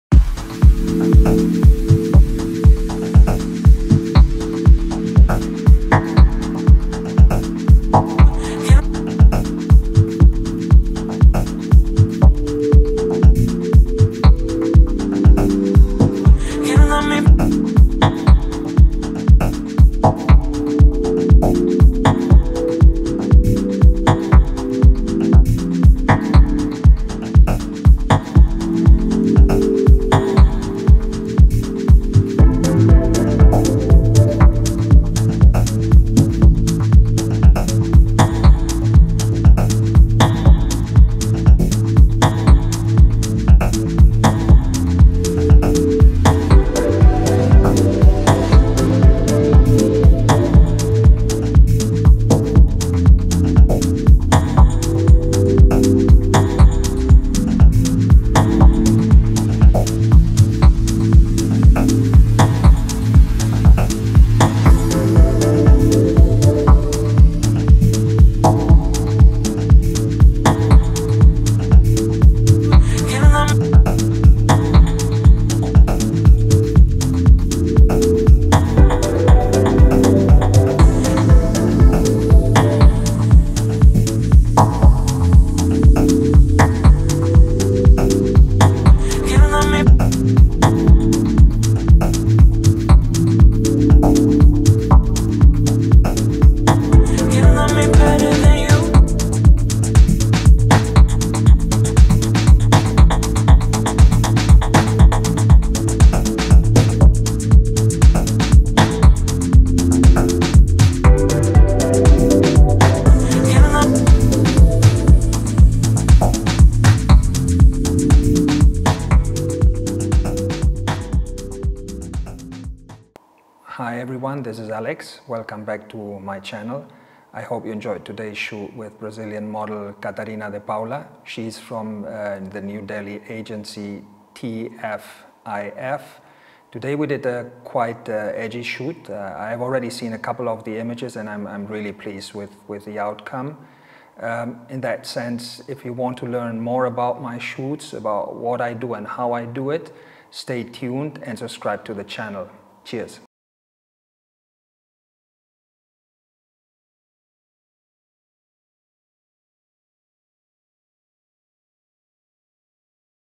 An Editorial Photoshoot | Behind the Lens with Catarina de Paula | By Alex F Buchholz
Welcome to the latest editorial shoot with stunning Brazilian model Catarina de Paula from TFIF Models.

For this shoot, we used as usual Profoto B1, B2, and B10 flashes.
As main camera, we used this time the Nikon D850, my brand new Sony A7RIV as well as the Canon EosR.
I edited the images on Eizo monitors as always, to achieve the best possible quality and color accuracy.

Photography:  Alex F Buchholz
Model: Catarina de Paula
Agency: TFIF Models
Stylist: Maitrayee Das
Makeup and Hair: Nidhi Thakur
Behind the Scenes: Shakool Das